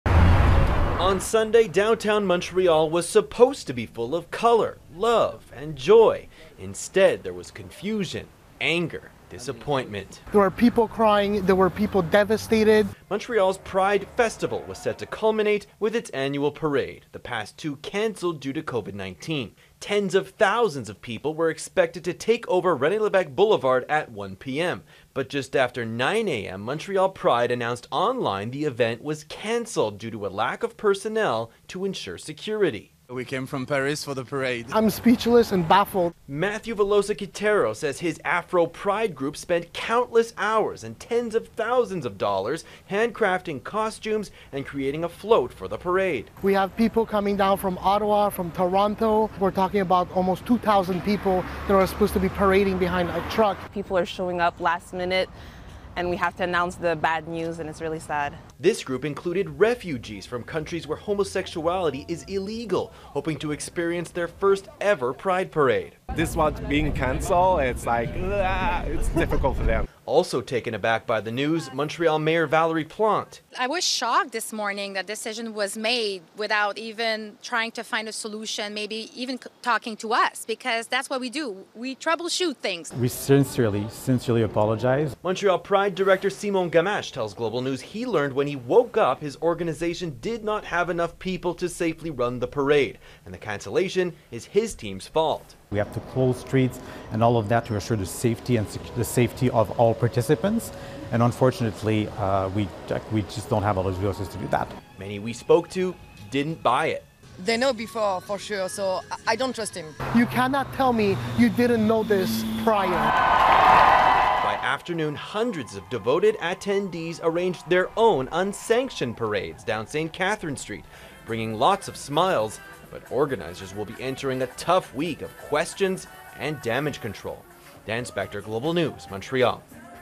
Montreal Pride parade cancelled hours before event due to lack of staff
Pride Montreal said the city’s iconic parade that closes the festival’s celebration of LGBTQ+ communities was cancelled on Sunday, citing security concerns stemming from a lack of volunteers for the event.

In a statement, organizers said the decision was made with the support of city police because there were not enough volunteers to ensure safety along the parade route.

But Mayor Valerie Plante says the city was not warned about the staffing shortage and could possibly have come up with a solution if it had been informed.

Dan Spector has more on the decision and the disappointment among parade participants and spectators.